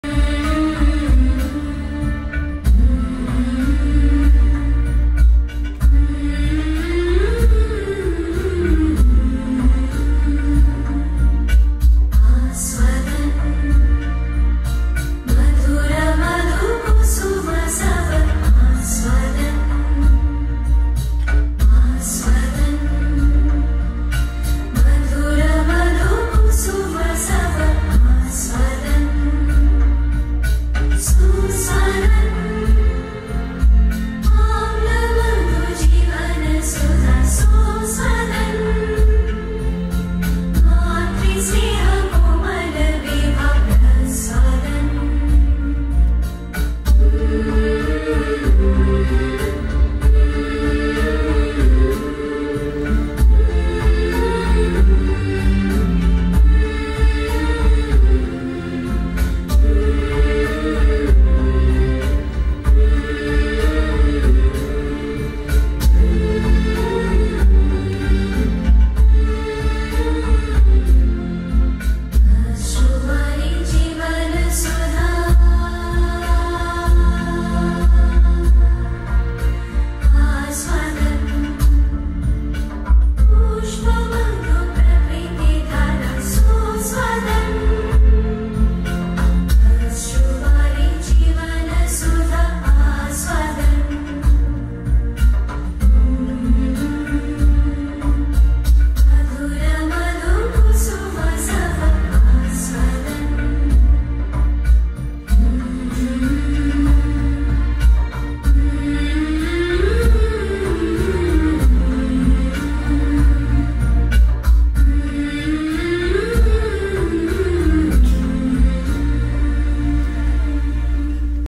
"Aswadham" A part of We The Living: Tanusree Shankar Dance Company: Uday Shankar Style
Choreography by Tanusree Shankar
Lights by Dinesh Poddar
Costumes by Abhishek Ray

Music Copyright: Sreenanda Shankar